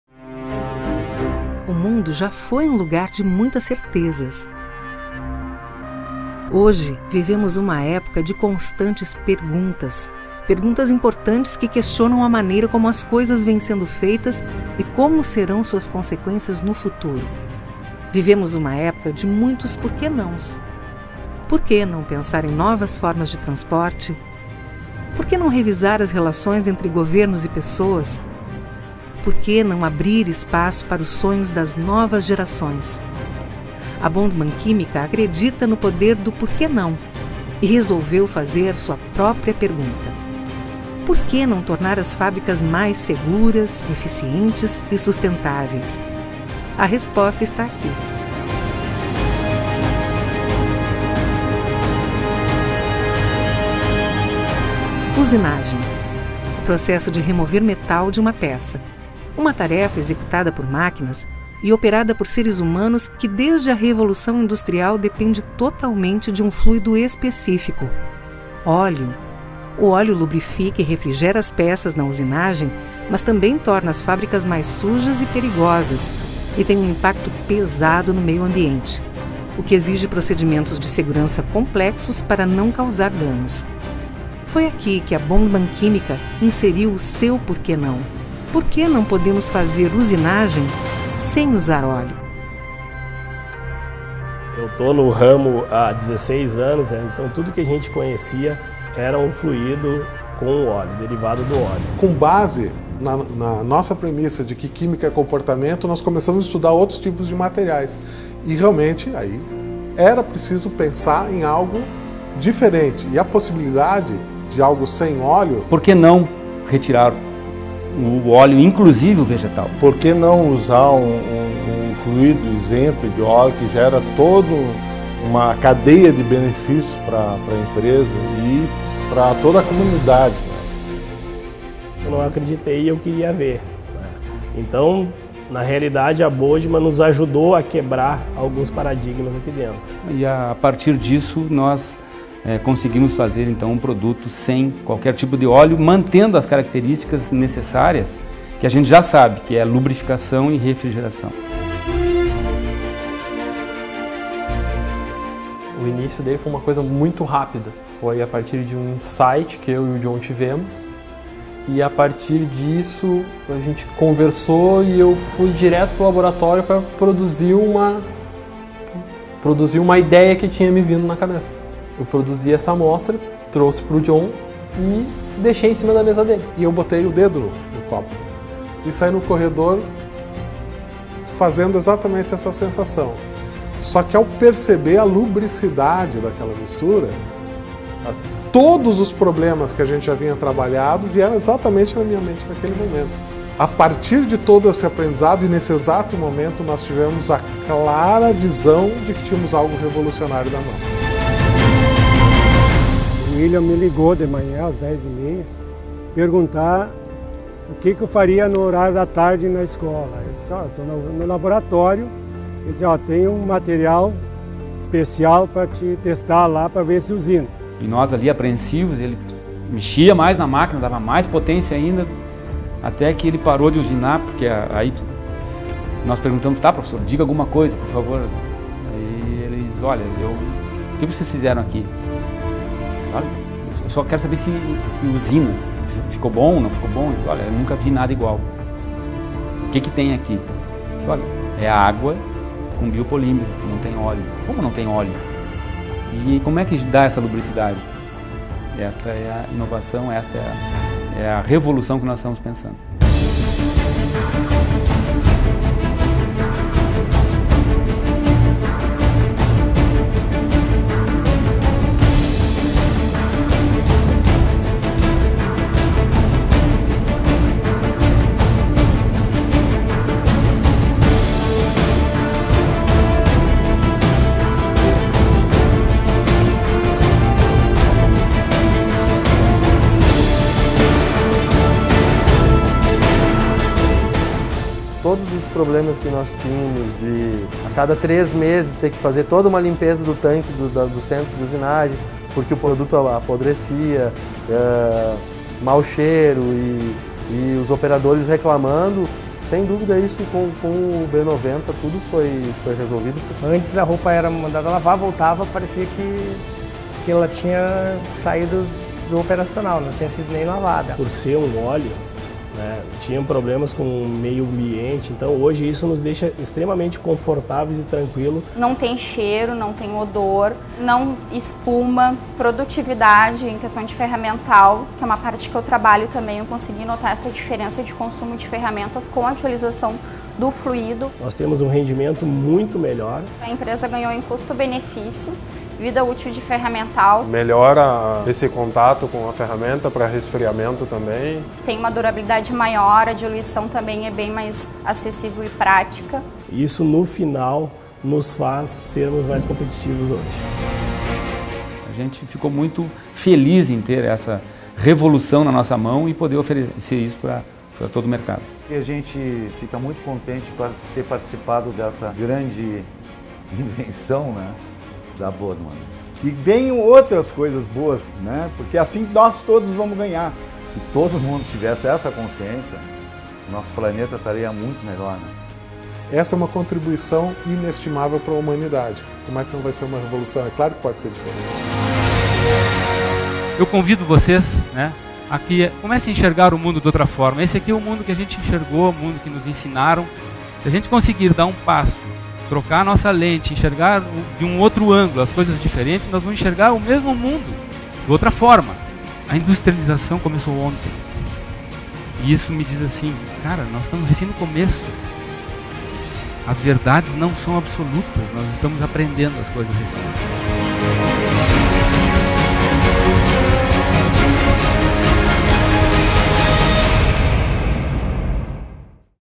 Bondmann video case - First Oil Free Machining Fluid
Check the full  history about the first oil free machining fluid, created by Bondmann Química. The brazilian company is  revolutionizing the entire industrial sector.